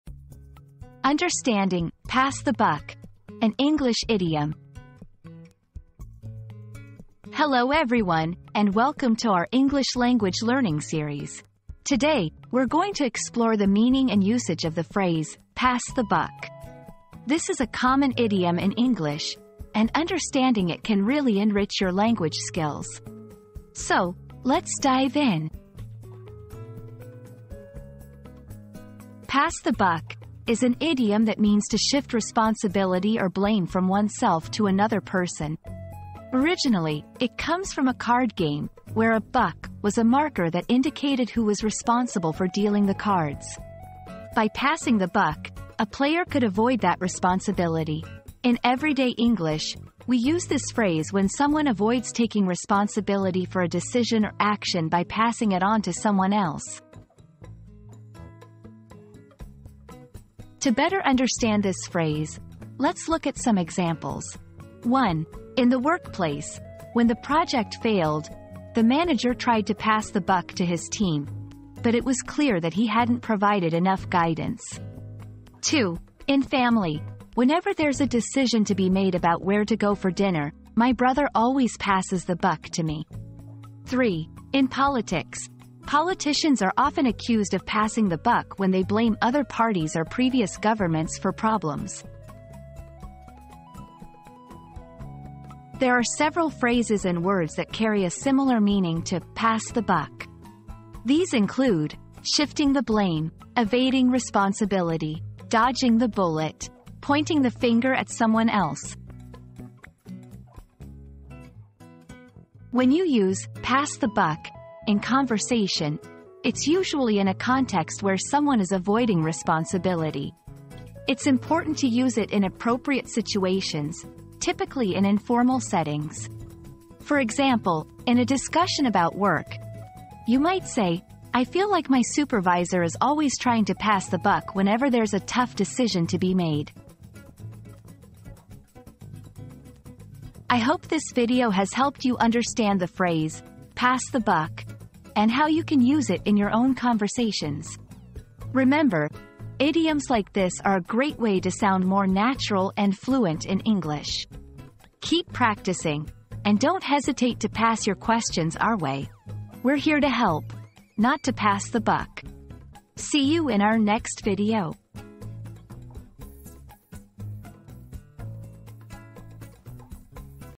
Understanding "Pass the Buck" - An English Idiom
Cracking the Code: Decoding the English Idiom 'Pass the Buck' • Unlock the meaning behind the popular English idiom 'Pass the Buck' in this engaging video. Discover its origins, usage, and how to incorporate it into your everyday conversations. Don't let this idiom stump you any longer!

00:00 • Introduction - Understanding "Pass the Buck" - An English Idiom
00:28 • What Does "Pass the Buck" Mean?
01:00 • Examples of "Pass the Buck" in Sentences
01:35 • Similar Phrases and Synonyms
01:52 • Using "Pass the Buck" in Your Conversations